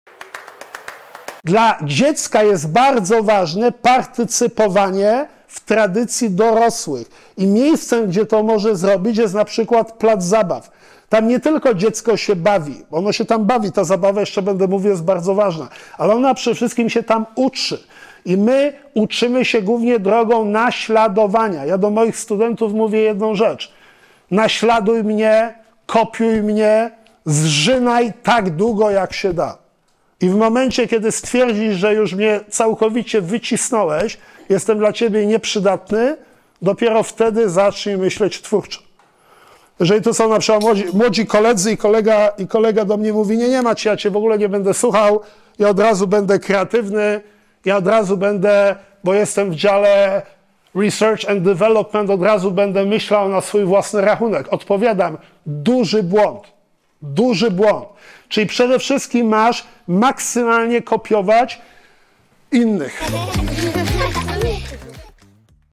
🟠🧠 Gdzie i kiedy rozwija się mózg? - Prof. Maciej Błaszak
Przedstawiamy drugie wideo z cyklu Buglo Experts Circle! Prof. UAM dr hab. Maciej Błaszak odpowiada na pytanie, gdzie i podczas jakiej aktywności mózg się rozwija.